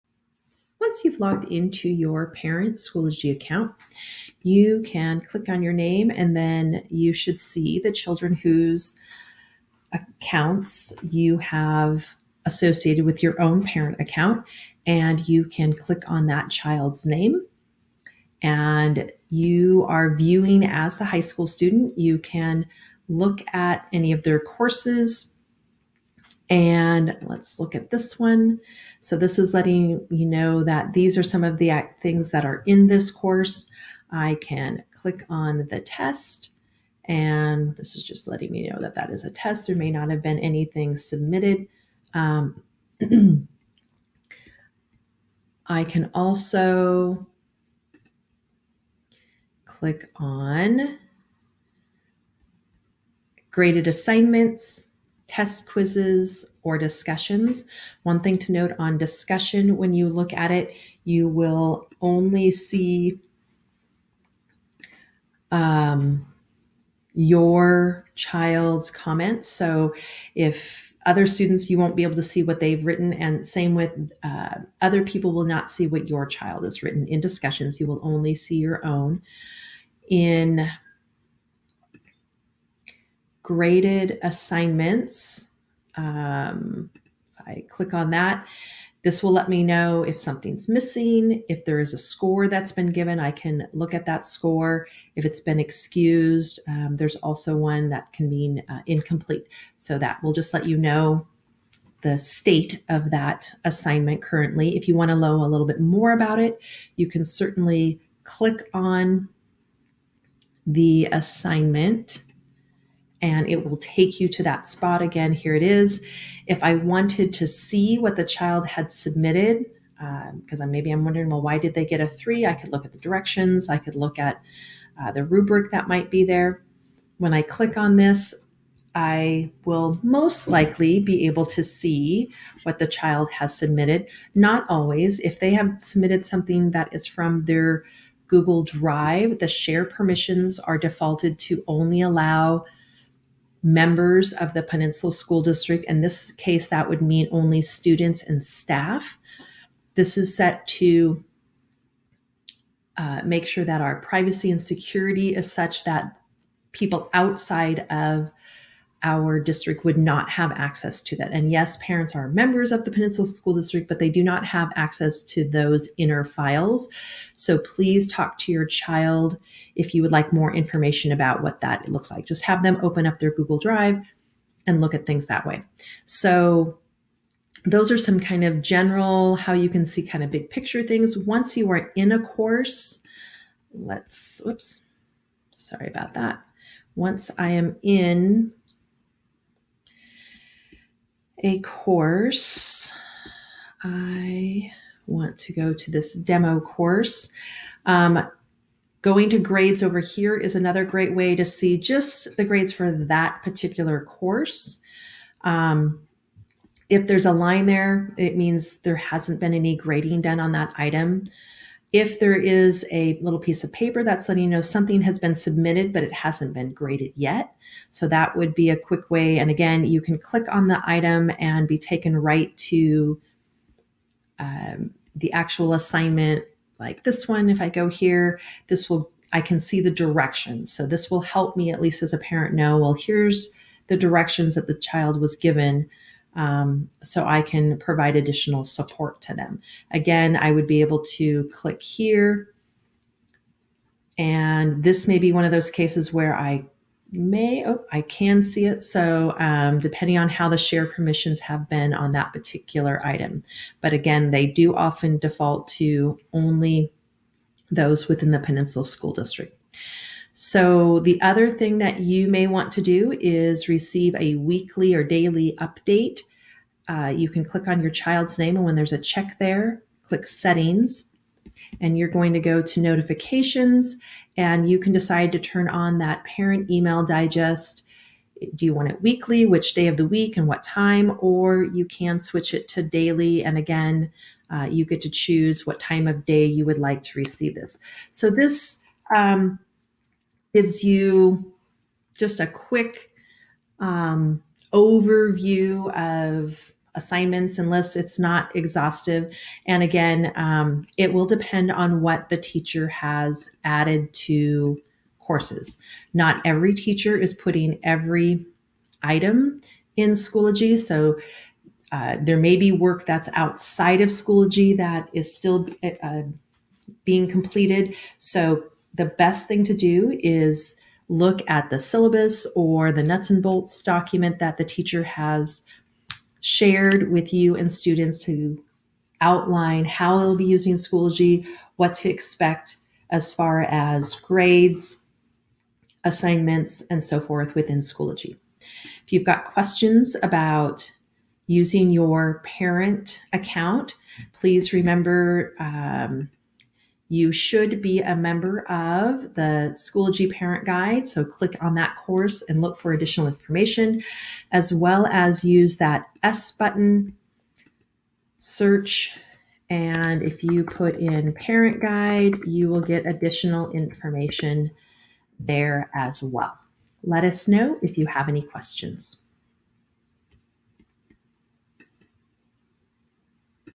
Family Supports | How Parents Check Student Progress in Schoology
This video shows parents how to check on student progress and grades in Schoology.

The video is part our Family Supports resources that support families with education tool resources utilized by Peninsula School District.

for more information and resources.